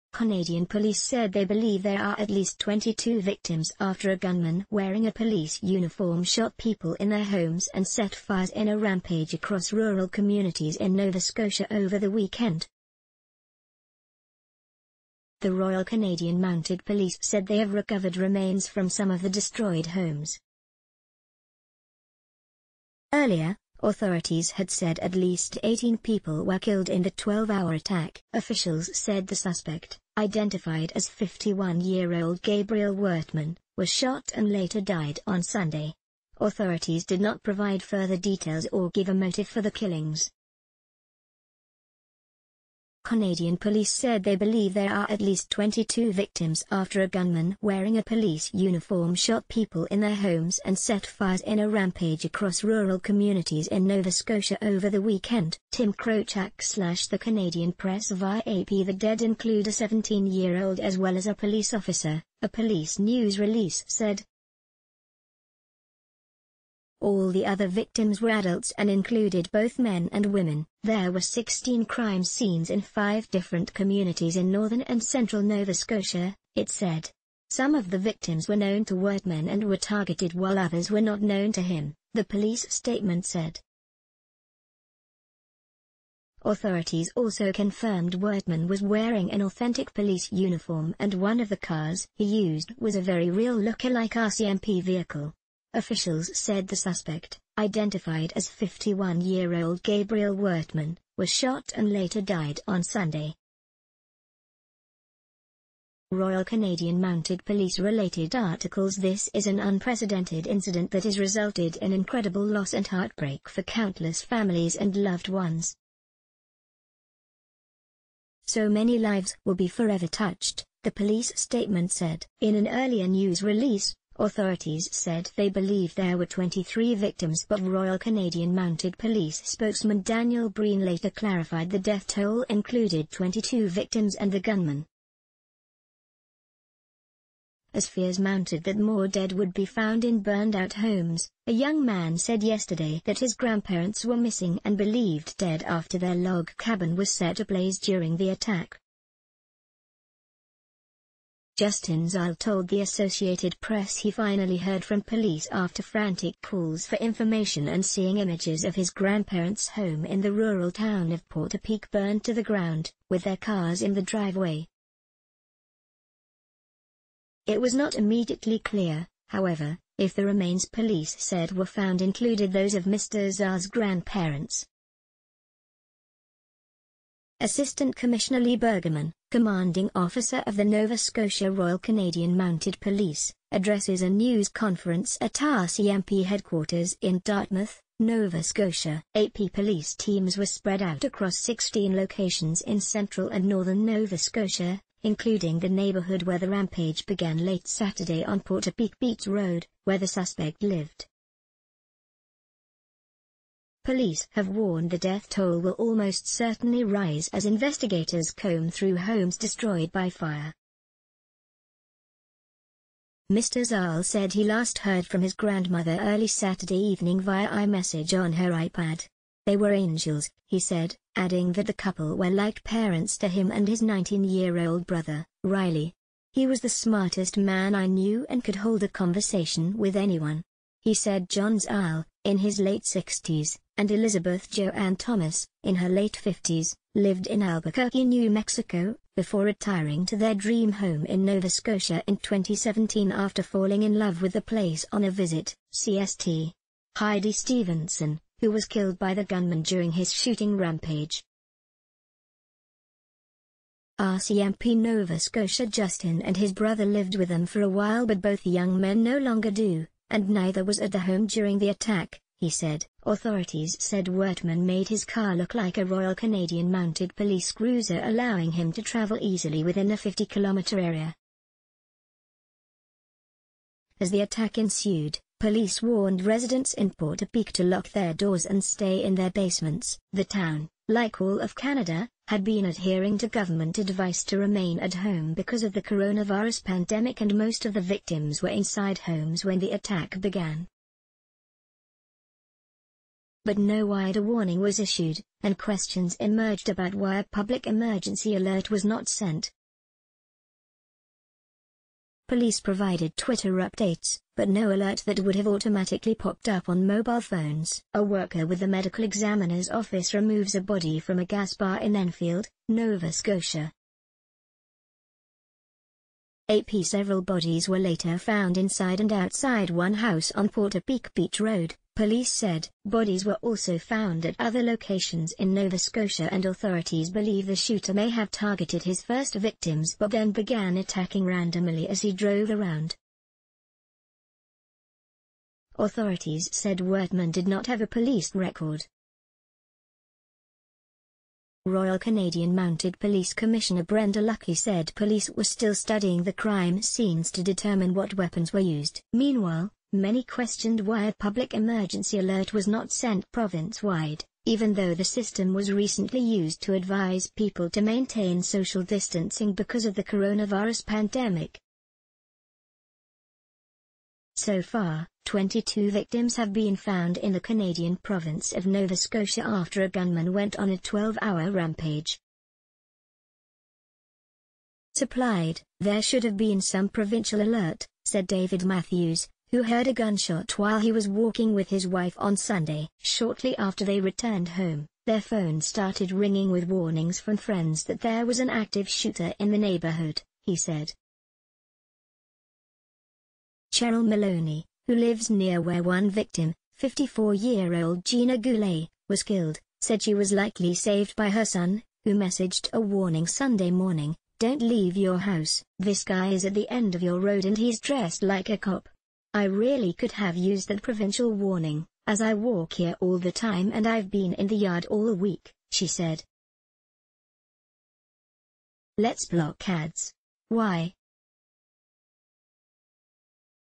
Canada victim death toll now at 22 with bodies still being recovered
Canadian police said they believe there are at least 22 victims after a gunman wearing a police uniform shot people in their homes and set fires in a rampage across rural communities in Nova Scotia ov...